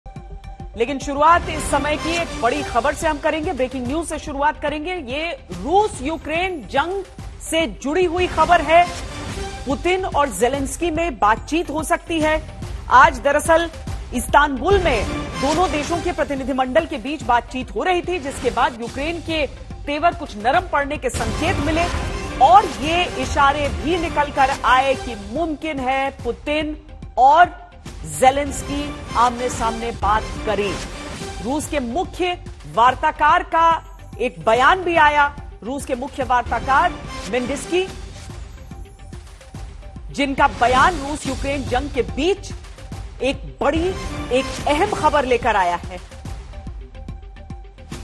Russia Ukraine War : इस्तांबुल में बातचीत के बाद दिखी नरमी | Latest Hindi News
Russia Ukraine War : यूक्रेन और रूस के बीच पिछले 34 दिन से भीषण युद्ध चल रहा है. रूसी हमलों के बाद पूरे यूक्रेन में तबाही का मंजर देखने को मिल रहा है. जंग के बीच राष्ट्रपति वोलोडिमिर जेलेंस्की ने दावा किया है कि रूस शहर के मेयर्स को किडनैप कर रहा है. कुछ को हम ढूंढ नहीं पा रहे हैं, कुछ को हमने ढूंढ लिया है लेकिन वे जिंदा नहीं हैं.